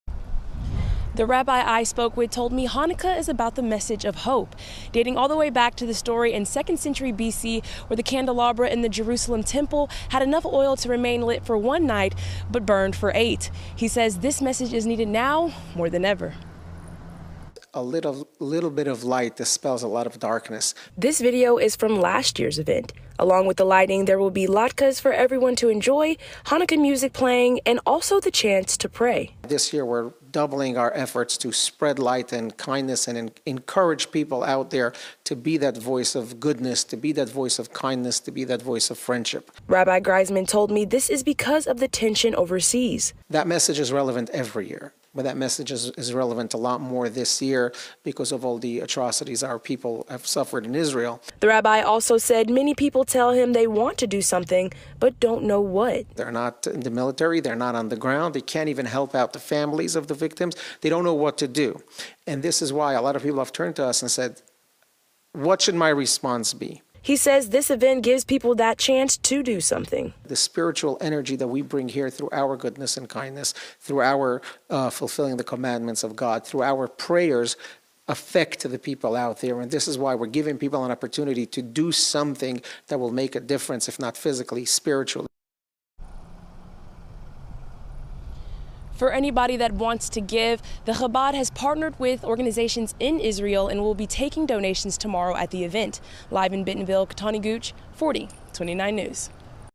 Bentonville Hanukkah celebrations planned for Thursday,
"A little bit of light dispels a lot of darkness." Rabbi Greisman said.